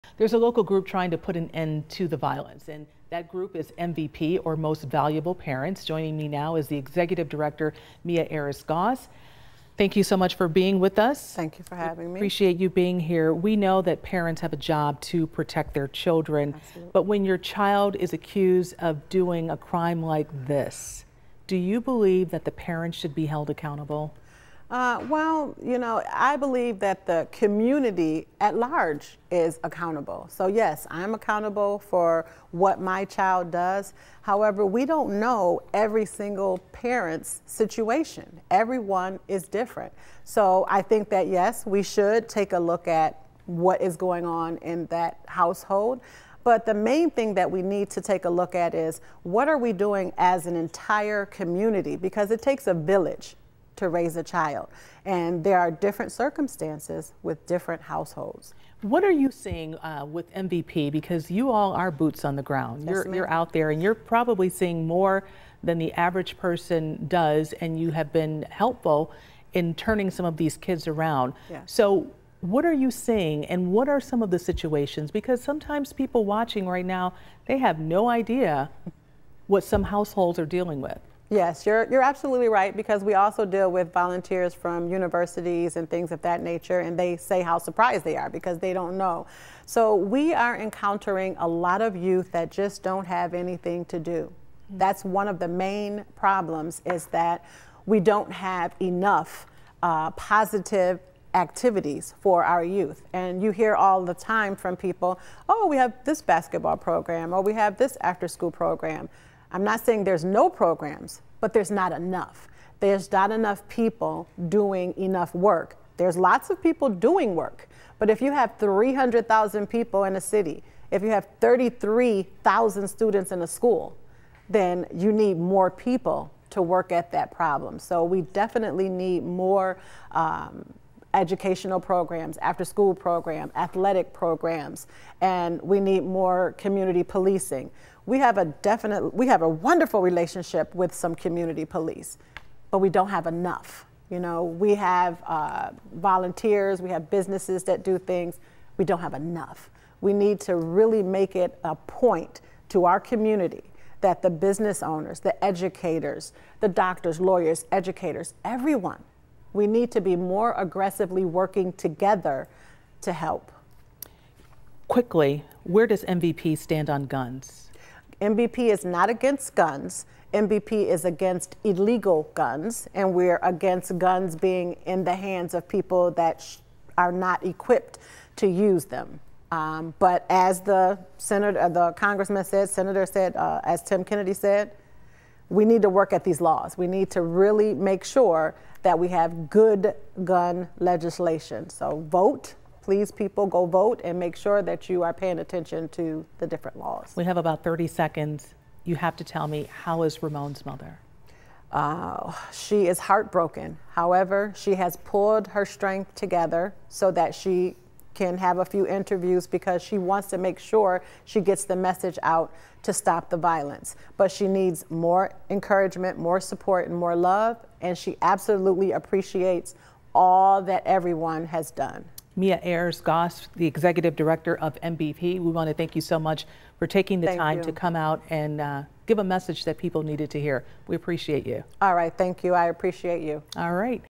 Tackling violence among teens
Most Valuable Parents executive director Mia Ayers-Goss talked with Claudine Ewing about tackling violence among teens.